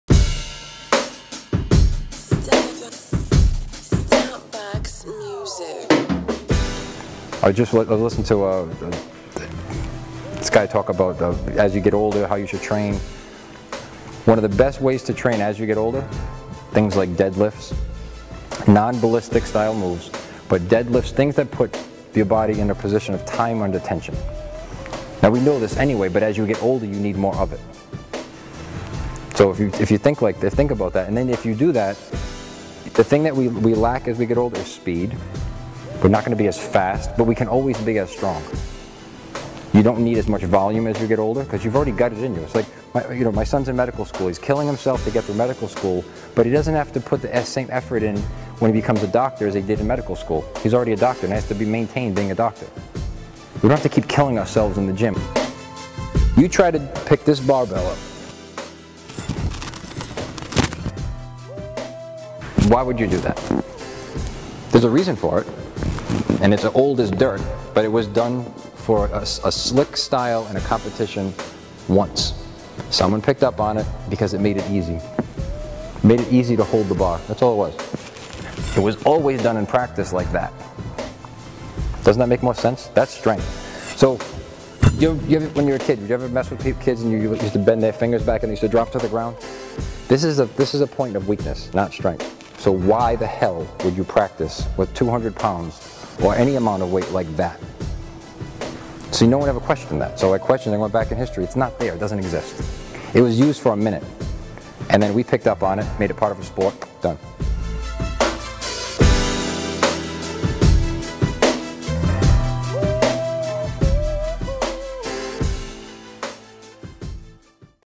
Develop and Discover, N.H. | inside scoop
Anthony is speaking to the trainers in a training course. here a little inside look.